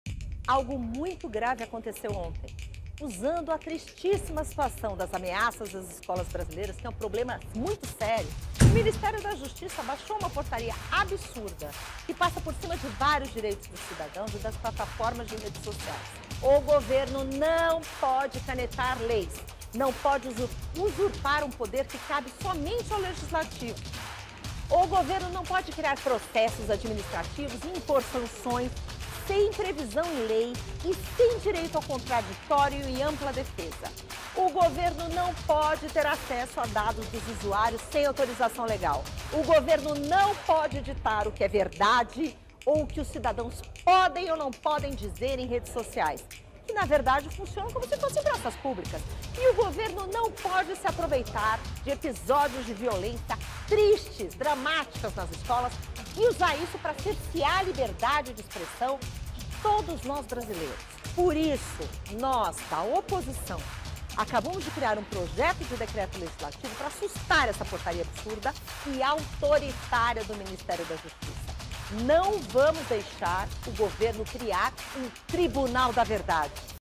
Para cada ação maluca do Governo, uma reação sensata da oposição!
Não vamos admitir que o Governo passe por cima do Legislativo e do Judiciário!